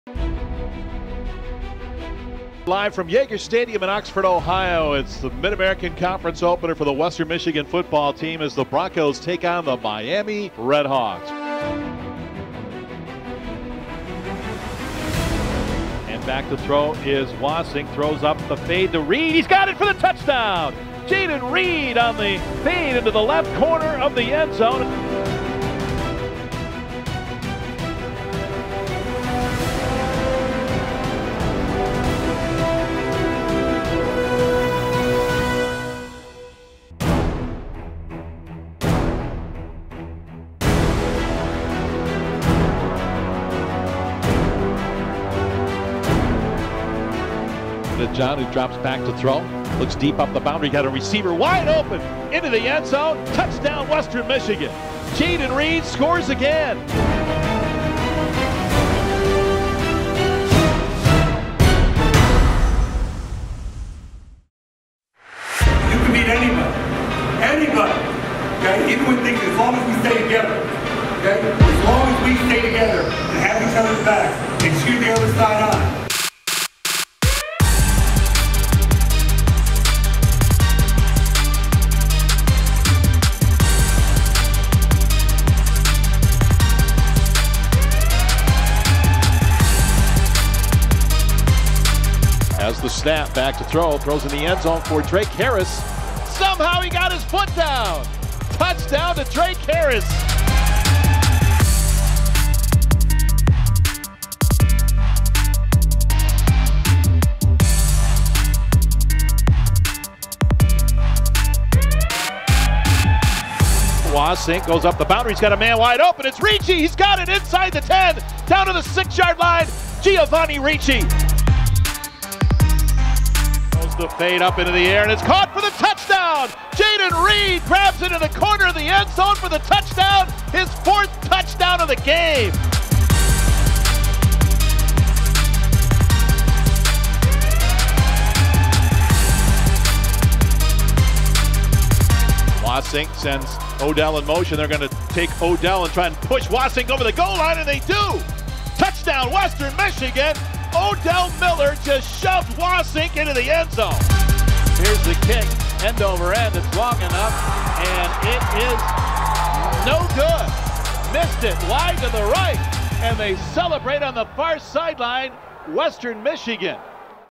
WMU Football vs Miami (OH) Highlights
Highlights from Western Michigan's win over Miami